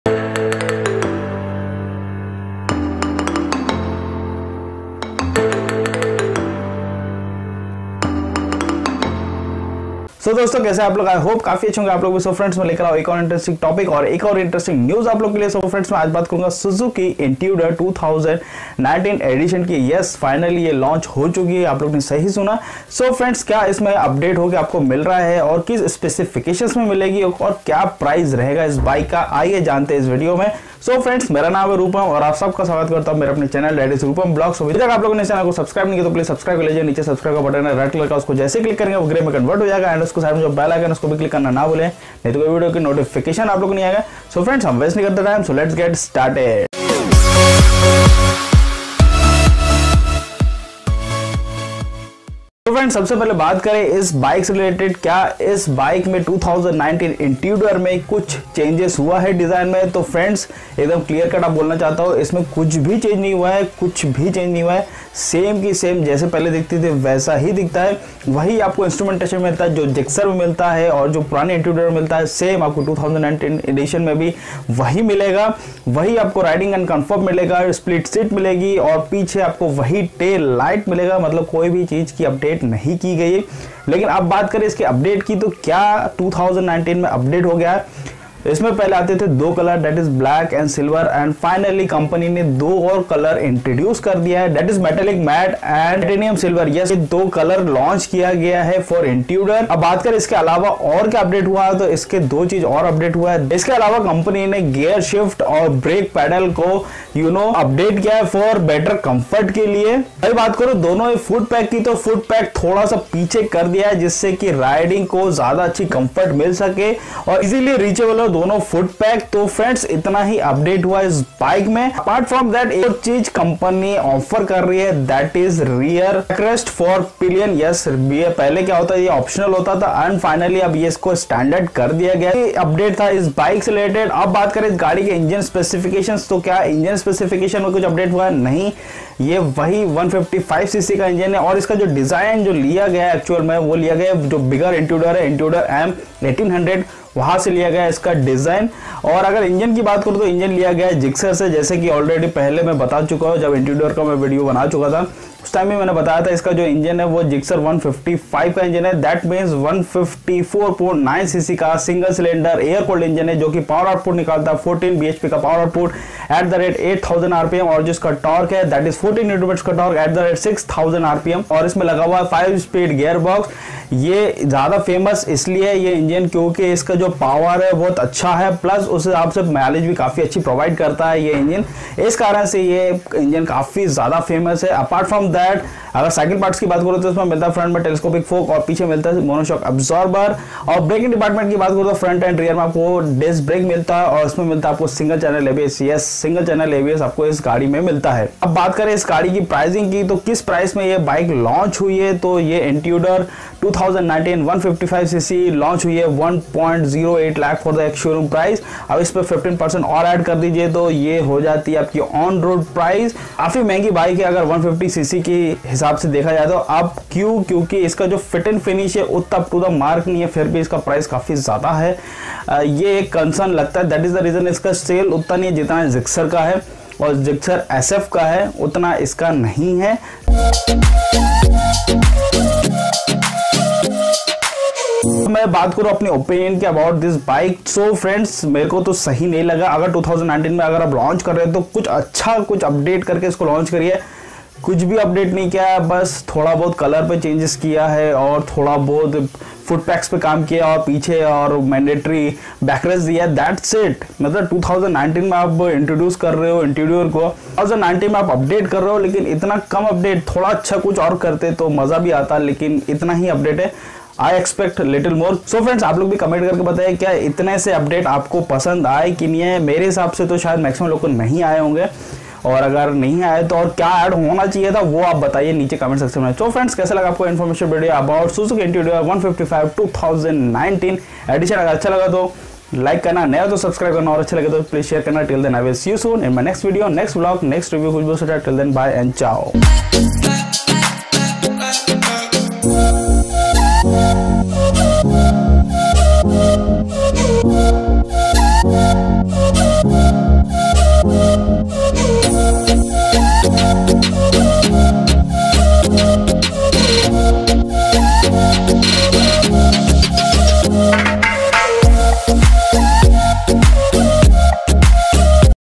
2019 Suzuki Intruder Launched at Rs 1.08 lakh ll Hindi
Suzuki has just rolled out a MY2019 update for the Intruder in India. The 2019 Suzuki Intruder has been priced at Rs 1,08,162 (ex-showroom Delhi) and comes with a new Metallic Matte Titanium Silver colour scheme.

Previous video:-

2019 KTM RC125 spied in India ll Rival the Yamaha YZF-R15 V3

I Used:-

Photron Tripod